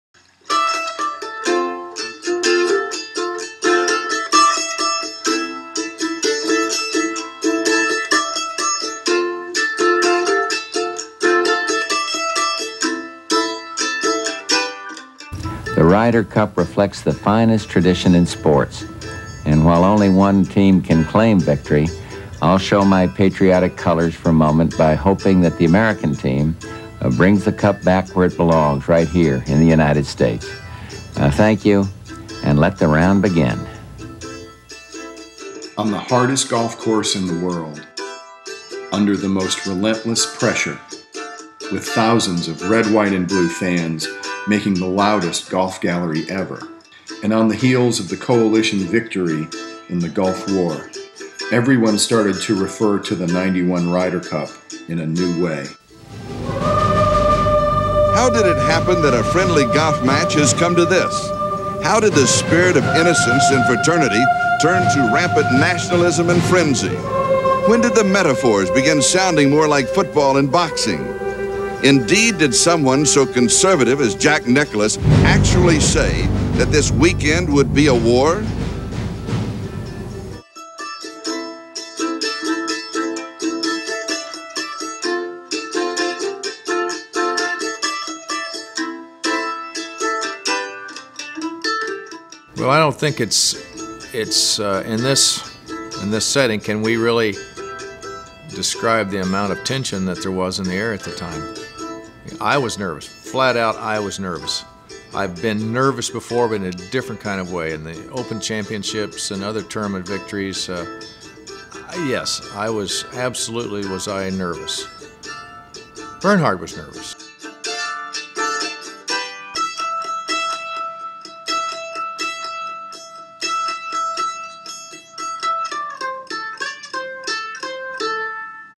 The War By The Shore by Curt Sampson: Official Book Trailer
The War by the Shore is the true story of the dramatic 1991 Ryder Cup at Kiawah Island when golf lost its manners (and, to some extent, its mind) and changed the competition forever. Written by New York Times bestselling author Curt Sampson.